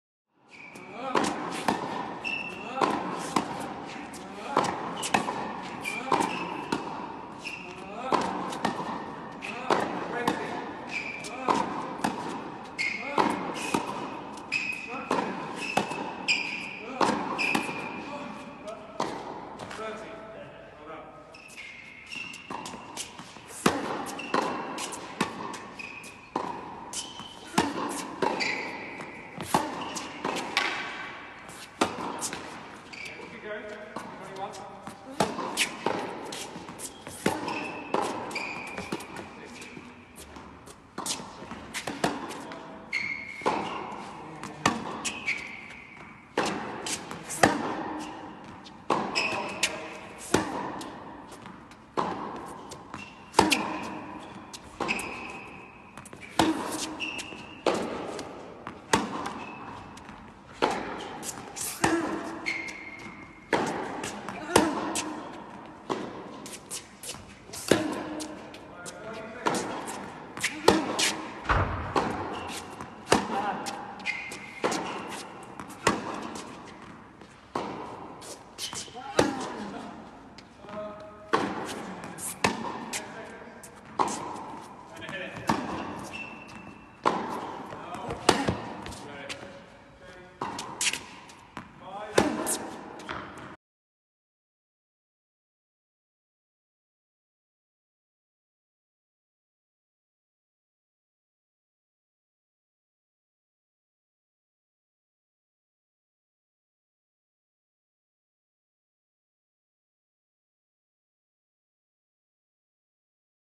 Back to Tennis | Jamie and Andy Murray | National Tennis Centre
First hit since lockdown restrictions lifted in England with Andy at the National Tennis Centre. Feels good to be back on court! 🎾🏆 🎬